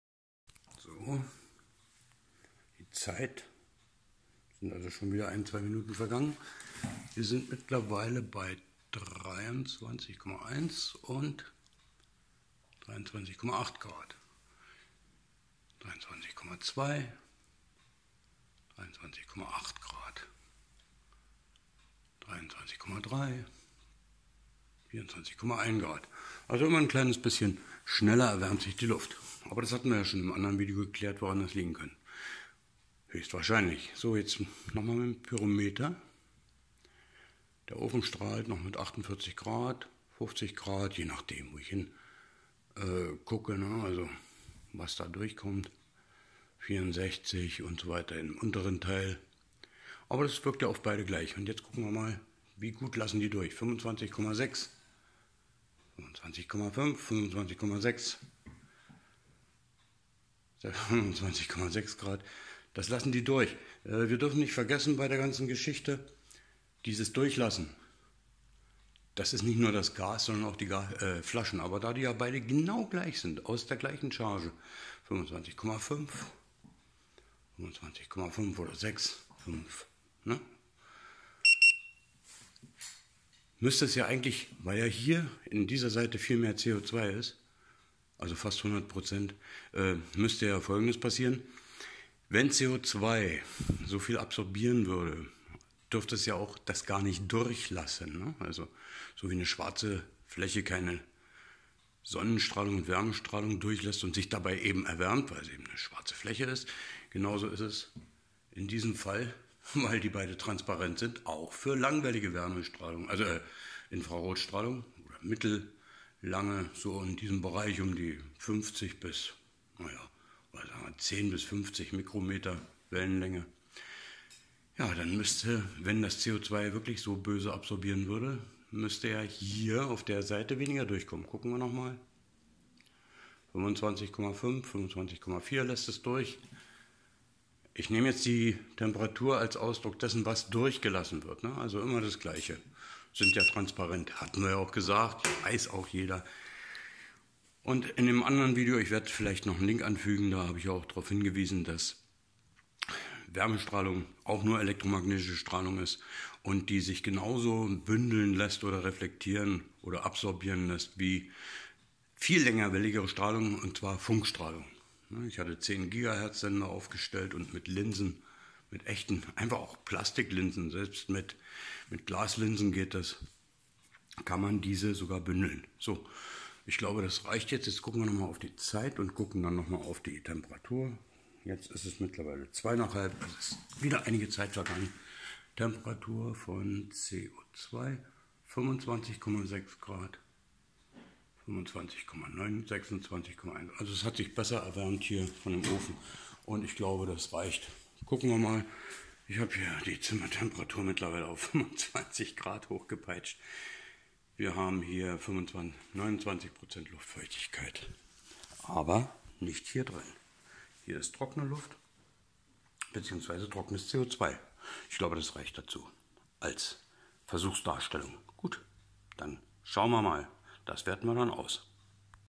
CO2-Lüge oder doch Treibhausgas? Teil 5_5, neuer Versuch mit langwelliger IR Strahlung, noch später
+++ Meinungsfreiheit im MaPhy-Forum +++ Bitte, helfen Sie uns Endlich wieder (Sapere aude!) CO2 und Luft sind bei gestiegener Temperatur immer noch gleich transparent. Die Gastemperatur steigt langsamer und beide Gase / Gasgemische sind gleich warm.

Dr. Suter schrieb an Prof.Dr. Lesch und kommentiert diesen Brief und den Klimawahnsinn hier (sehr sehenswert)

Björn Köhler

zzzzz_intern